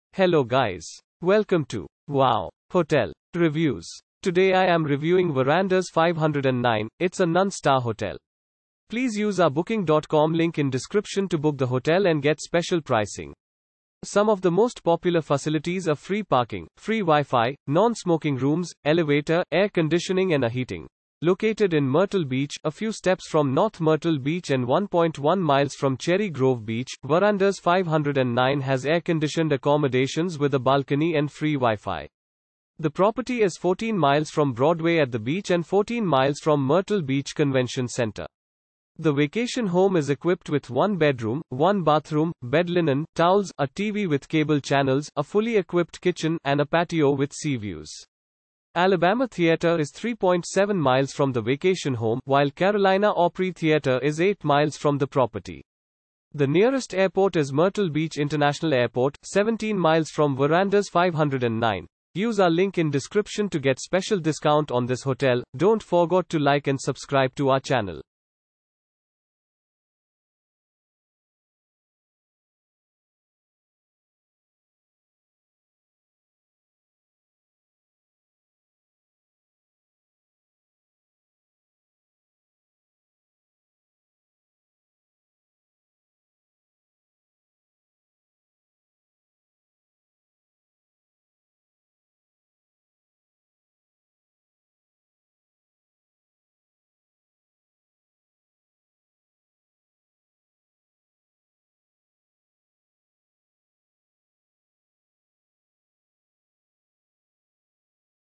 Verandas 509 Review - Myrtle Beach , United States of America
Verandas 509

Located in Myrtle Beach, a few steps from North Myrtle Beach and 1.1 miles from Cherry Grove Beach, Verandas 509 has air-conditioned accommodations with a balcony and free WiFi. The property is 14 miles from Broadway at the Beach and 14 miles from Myrtle Beach Convention Center.
The vacation home is equipped with 1 bedroom, 1 bathroom, bed linen, towels, a TV with cable channels, a fully equipped kitchen, and a patio with sea views.
Alabama Theater is 3.7 miles from the vacation home, while Carolina Opry Theater is 8 miles from the property. The nearest airport is Myrtle Beach International Airport, 17 miles from Verandas 509.